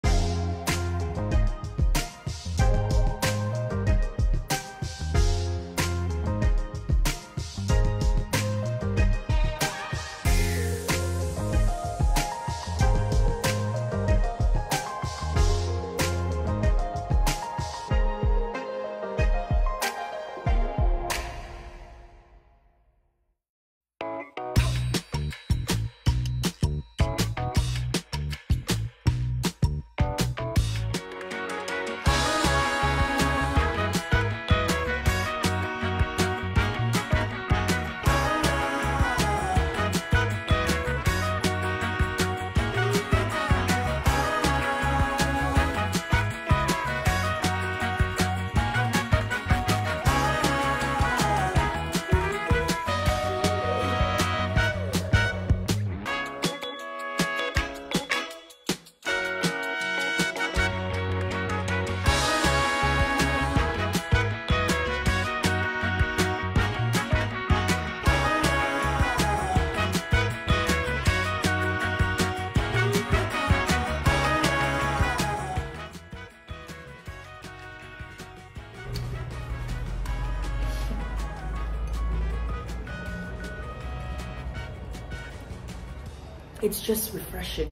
Homemade pineapple juice|No blender or juicer |Pineapple peels|Sierra Leone
Pineapple juice are delicious but can be quite expensive and can be full with perseverance.Make your own delicious  and healthy pineapple juice at home.